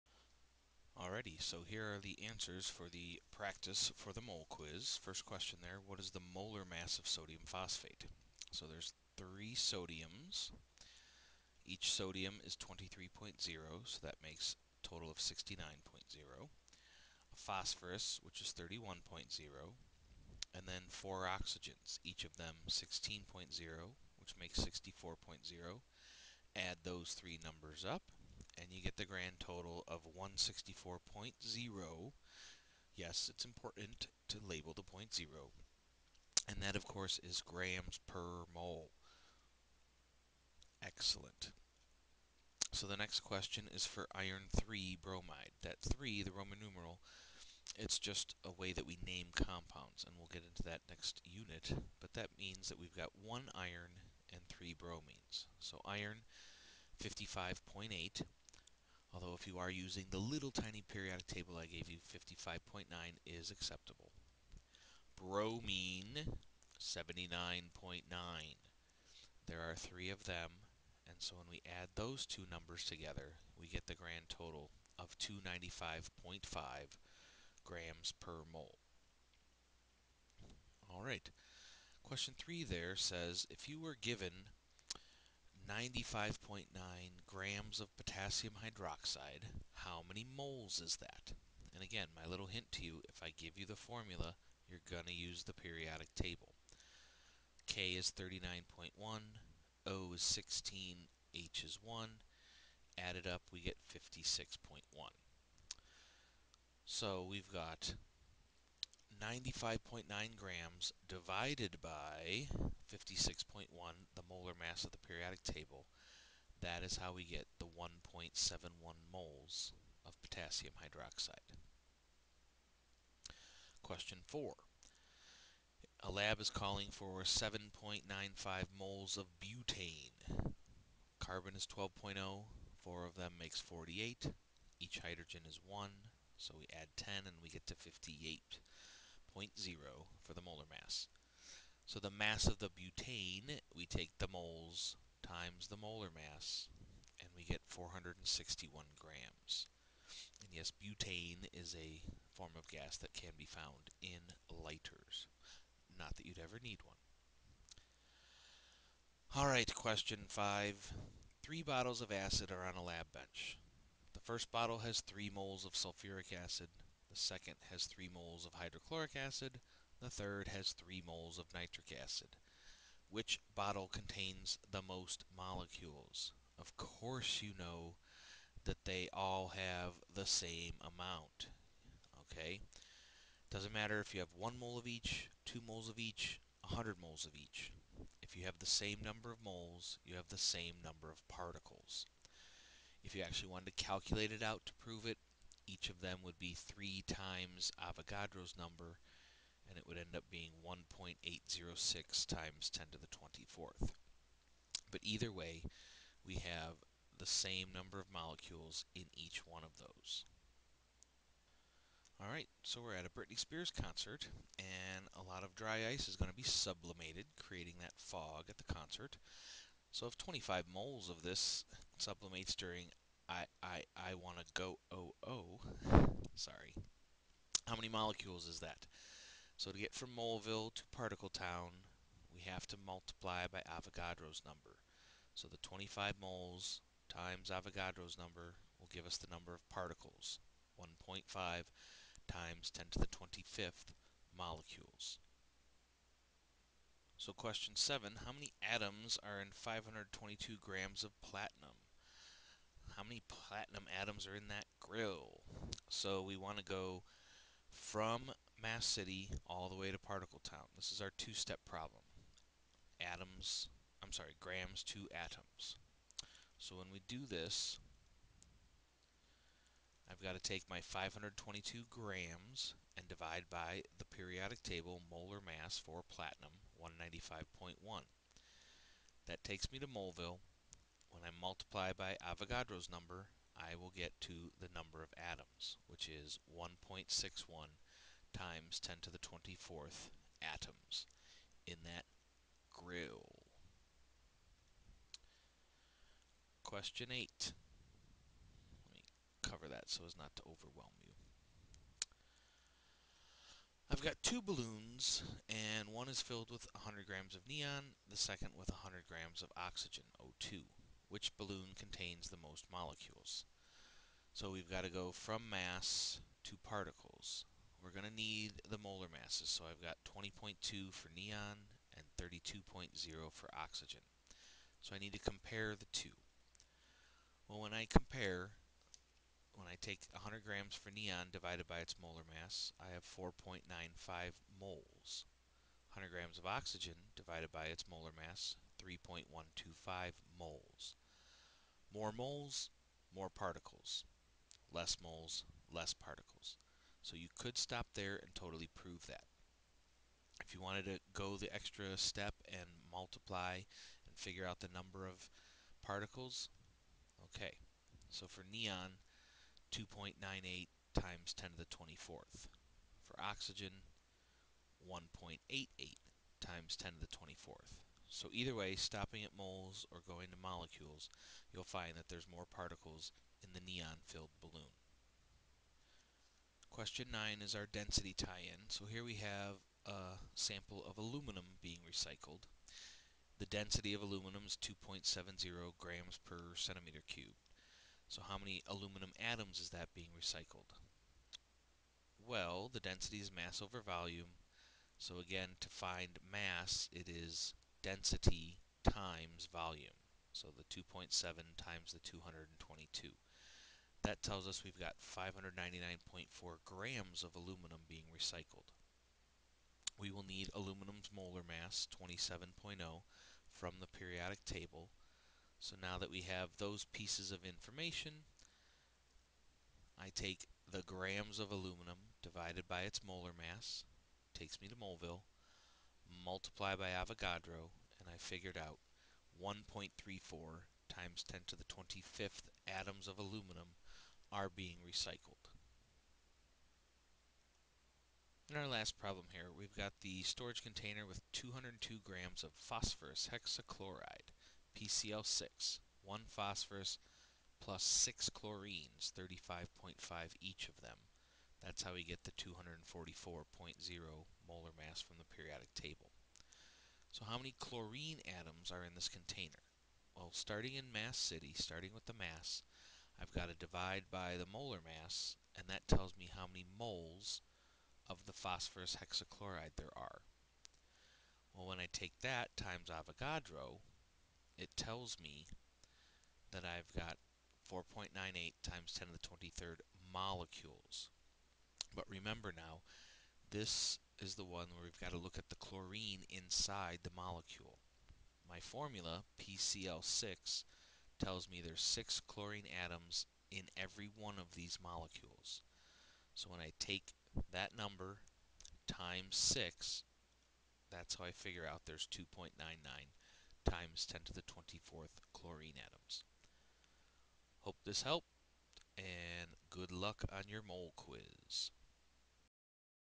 Practice for the Mole Quiz Answers
Answers to the Practice Mole Quiz for my General Chemistry class.  Molar mass and mole conversions using molar mass and Avogadro's number are the focus.